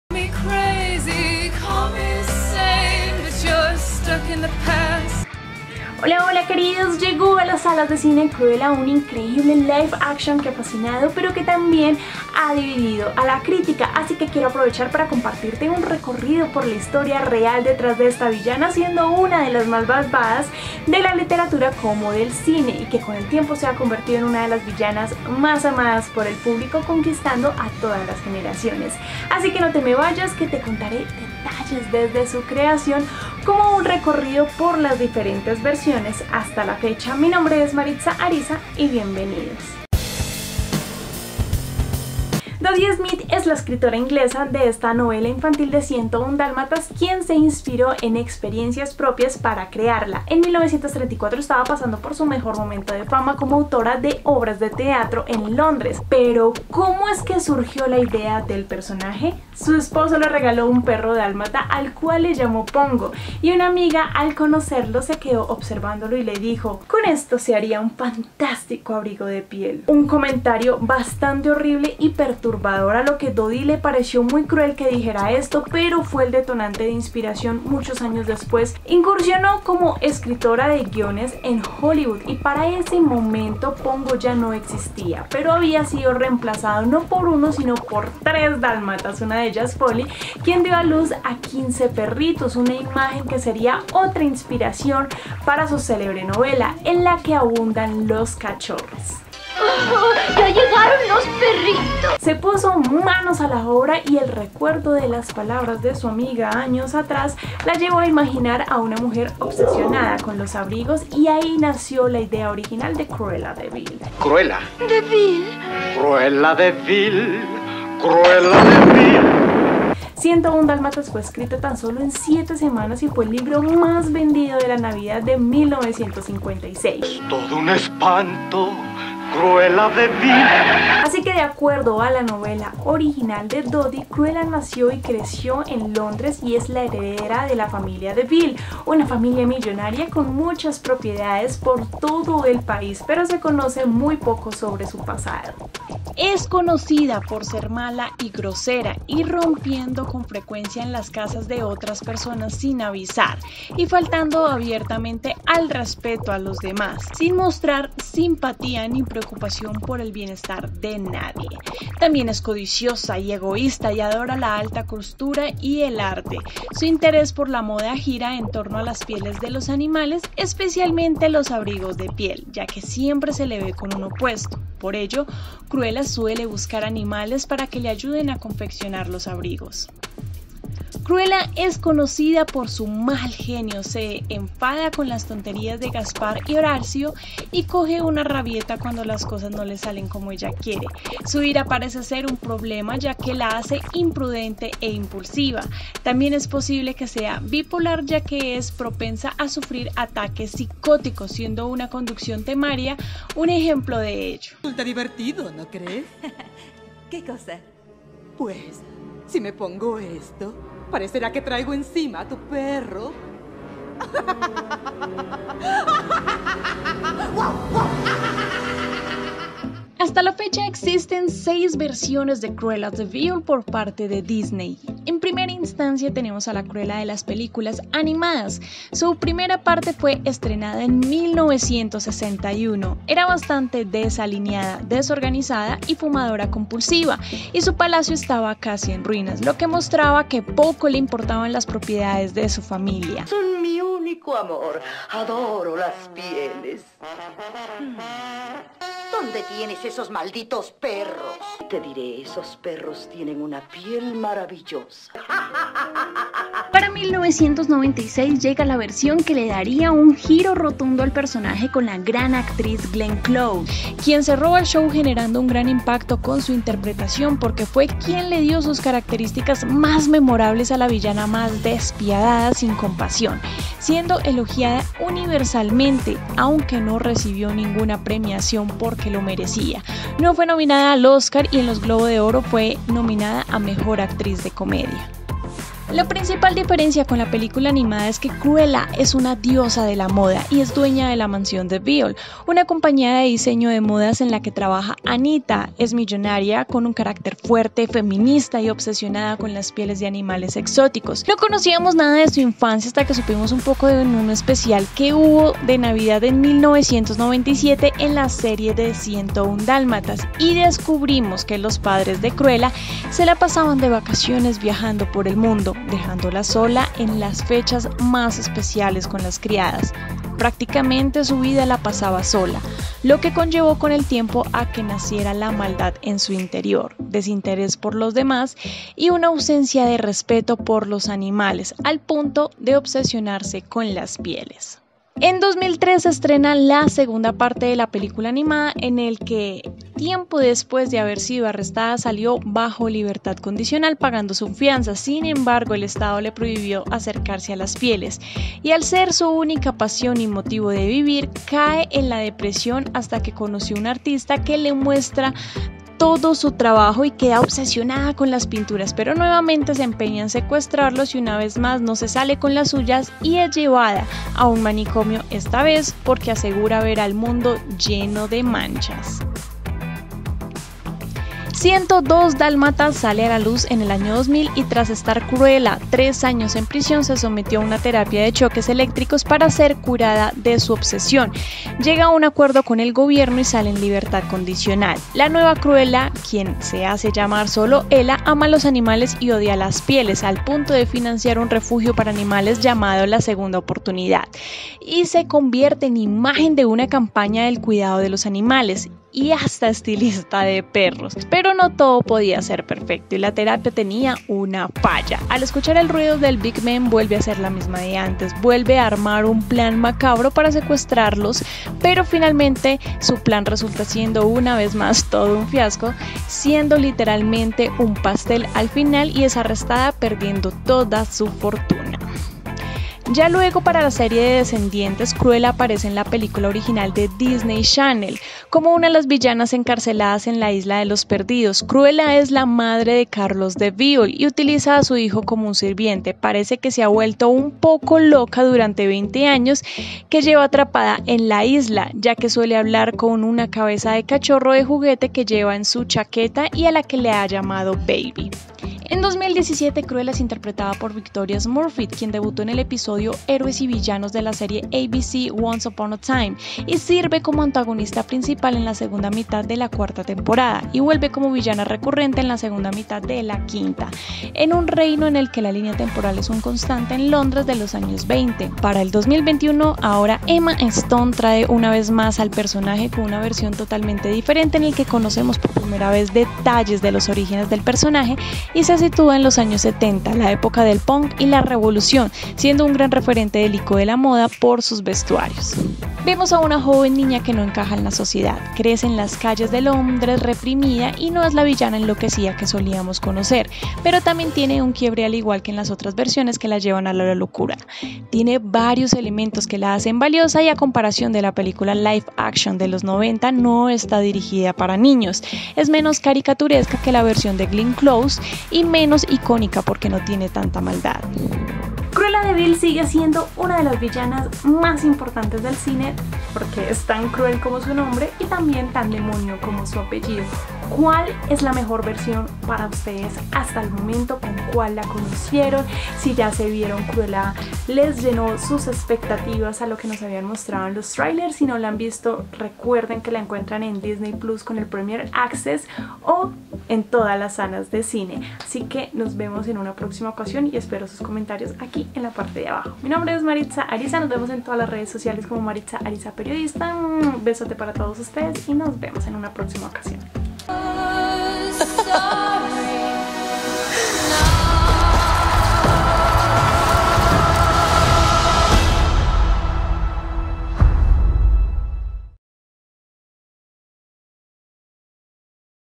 🖤 CRUELLA DE VIL historia y evolución del DEMONIO HECHO MUJER - Maritza Ariza
Si aún no te has inscrito a DISNEY PLUS LATINOAMÉRICA ingresa a este y podrás crear la cuenta.

¿Cómo surgió la idea del personaje? Vamos a ver el origen e historia de una de las mejores villanas de Disney, CruellaDeVil , el mejor live action de Disney.

Dodie Smith es la escritora inglesa de la novela infantil 101 dálmatas quien se inspiró en experiencias propias para crearla. En 1934 estaba en su mejor momento de su fama como autora de obras de teatro en Londres.

Ya pueden ver Cruella en cines o en Disney Plus Premiere Access, un increíble live action que ha fascinado pero también que ha dividido las críticas, así que quiero compartirles un recorrido por la historia real detrás de esta villana siendo una de las más malvadas de la literatura como el cine y con el tiempo se convirtió en una de las villanas más amadas por el público conquistando a todas las generaciones.

CRUELLA historia y evolución del DEMONIO hecho mujer - Maritza Ariza